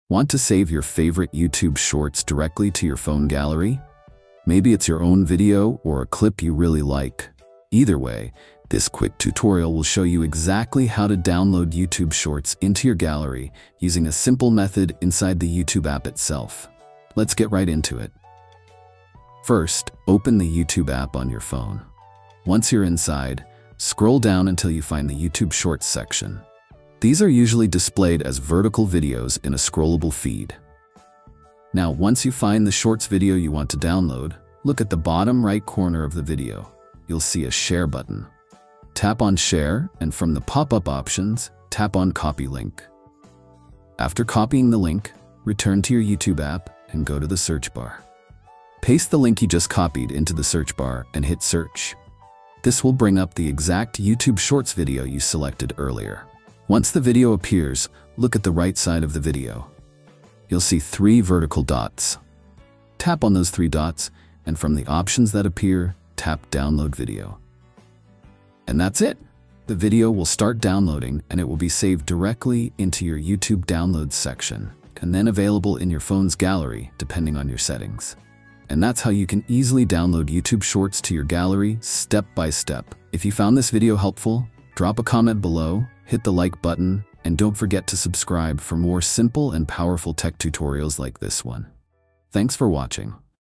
How to Download YouTube Shorts Video to Gallery | Save Shorts Directly to Your Phone
In this video, you’ll learn how to download YouTube Shorts directly to your phone gallery in just a few simple steps.
Whether it’s your own content or a short video you love, this step-by-step guide shows you exactly how to copy the link, find the video, and save it straight to your device.

What you’ll learn in this video:

How to find and copy the link to a YouTube Shorts video

How to locate the video using the link in the YouTube search bar

How to use the three-dot menu to download the video

Where to find the downloaded video in your gallery

Works for Android and iPhone users
No third-party apps required
Quick, safe, and easy to follow